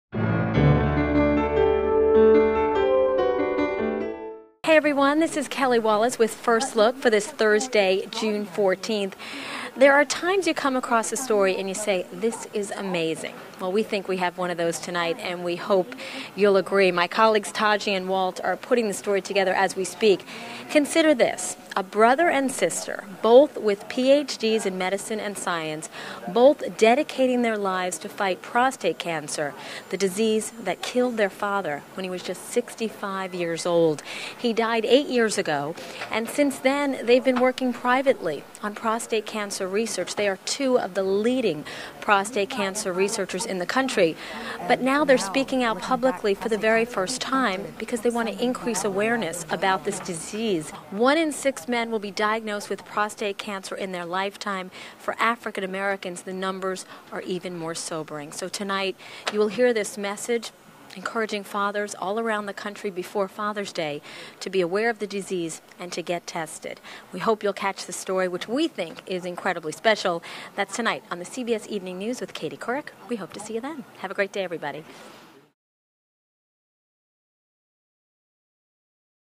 First Look: Fighting Prostate Cancer (CBS News)
Kelly Wallace gives a preview of her story about a brother and sister who have teamed up to find a cure for prostate cancer. (CBSNews.com)